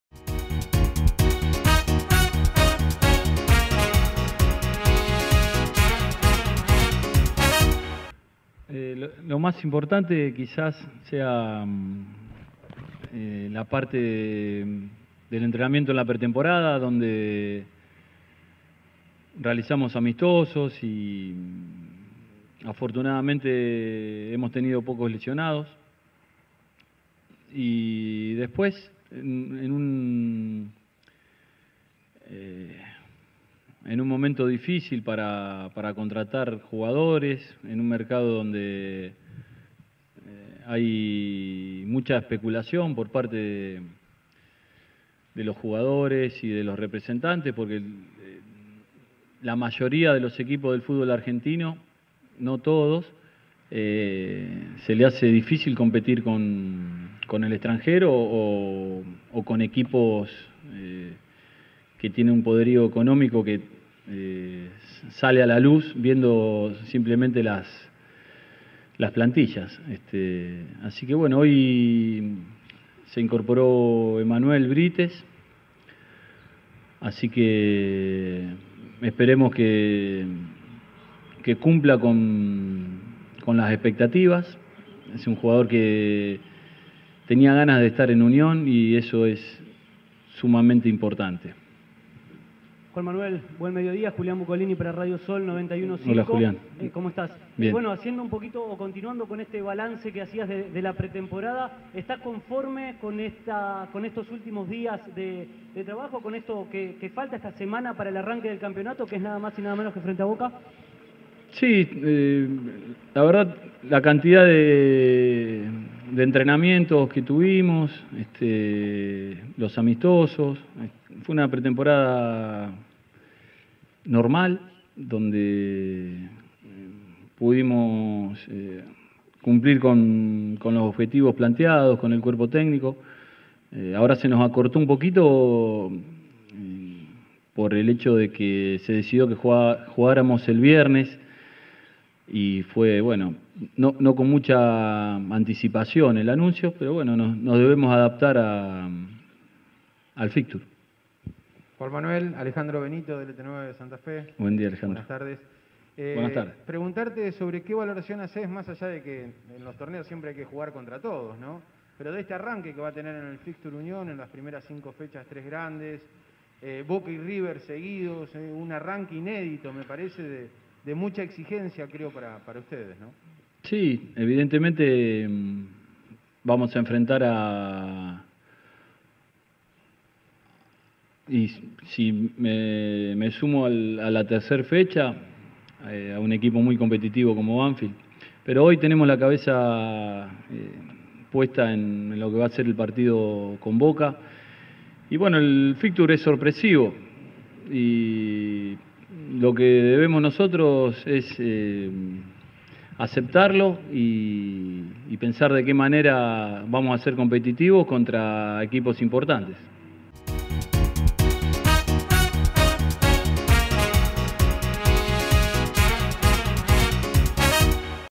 Juan Manuel Azconzábal
El DT juninense de Unión de Santa Fe habló en la previa al partido del viernes frente a Boca Juniors.